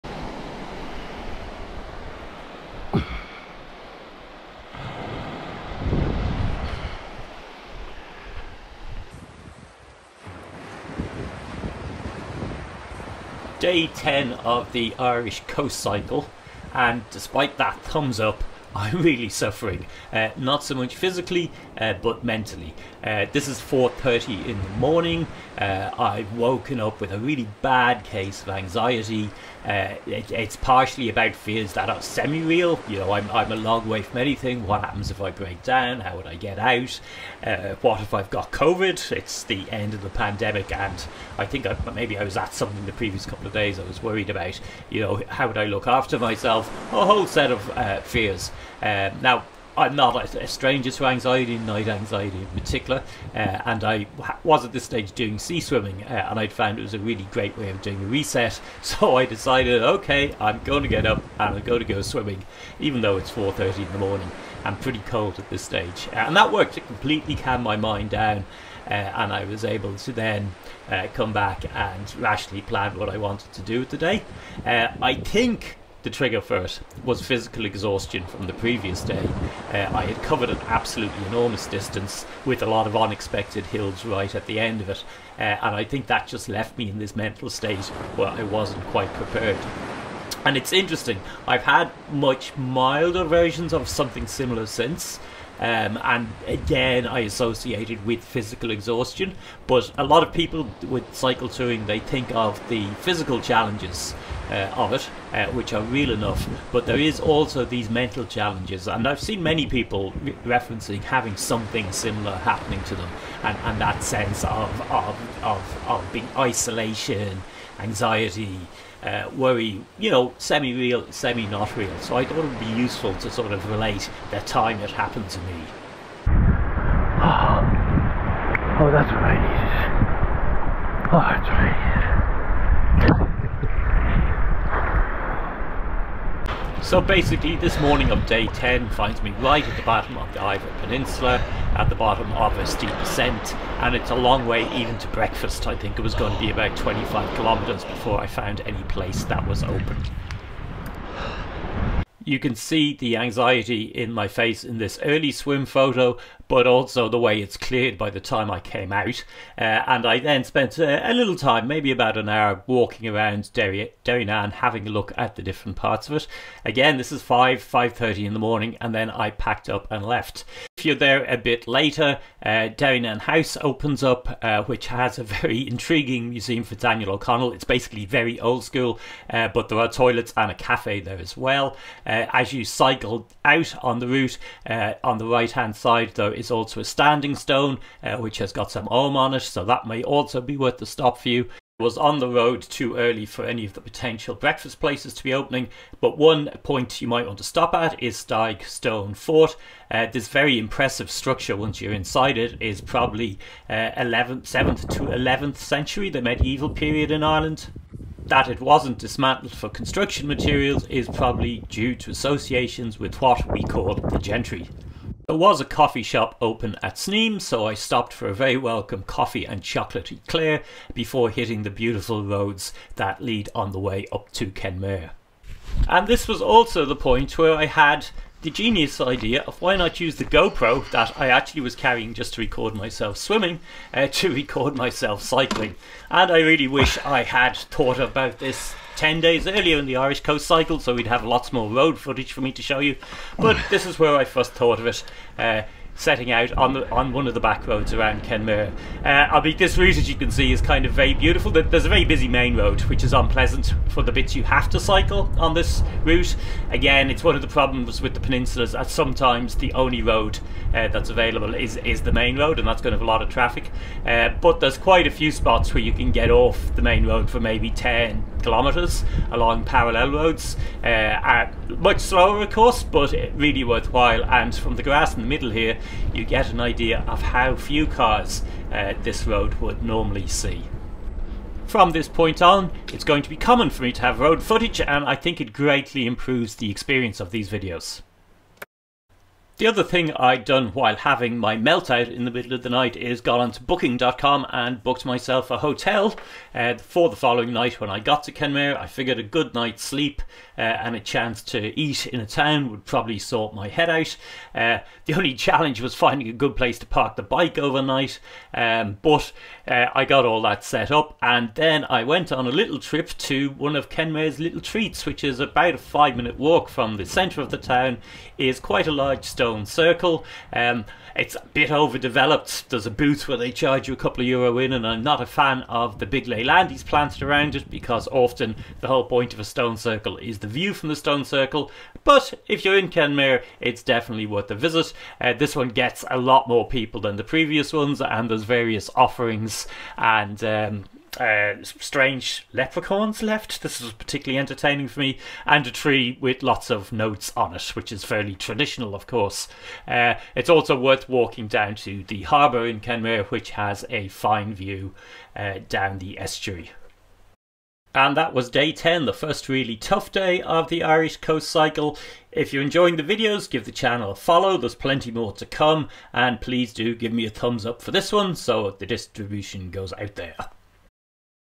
Cycle touring the Ring of Kerry - Derrynane to Kenmare, day 10 of bike packing Irelands coast
Day 10 found me waking far too early on Derrynane beach and going for a 4.30am swim to calm my racing mind. I open this diary of my Irish Coast Cycle with discussion the huge psychological anxiety hit of that night, the worst I've had to overcome on my touring to date and where it came from.

After mow swim I walked over the to the ruins of the abbey and then set off, stopping just down the road where if your on a bicycle you can access a standing stone with ogham.  This stone is a couple of km from Derryanne on the coast road which later climbs back to the main road but you can also walk through it via the Derrynane house grounds.

For much of the route to Kenmare the main road is the only option but there are a few points where the network of side roads joins up and make for very much more pleasant cycling.  Otherwise try and do this early in the morning and avoid the afternoon when the tour coaches that set out from Killarney will be catching up-with you.  The mostly follow that direction so its an argument for doing this section in the opposite direction to mine although I was at Kenmare ahead of them

Starting in the morning at the end of any of the peninsulas also works to minimise heavy traffic as the huge tourist buses are starting at the top and so shouldn’t catch you if you complete the trip by close to lunchtime.  This is a reoccurring issue in several peninsulas in Ireland and also at the Cliffs of Moher

Where you can take parallel back roads they are beautiful and it's also with diverting to the circular stone fort at Staigue a short distance off the route. This is impressive in itself but also has a fine view from it.

When I awoke at 4.30 I had also decided to book myself into a hotel the following night in Kenmare so I could get a good sleep, feed and fully recharge the batteries. I had intended to do a hotel/B&B every 2-3 nights but not slept much the previous night so badly needed a catch up.

There is a  stone circle is in Kenmare & it has a 2 euro entrance fee - unfortunately it’s been surrounded by hedging blocking the views to the surrounding mountains. This is poor as stone circles seem to have been located for the views and with some the shape of the top of each stone seems to echo the outline of a mountain of hill behind it. Its worth a visit though as is the short walk down to the harbour on the edge of town.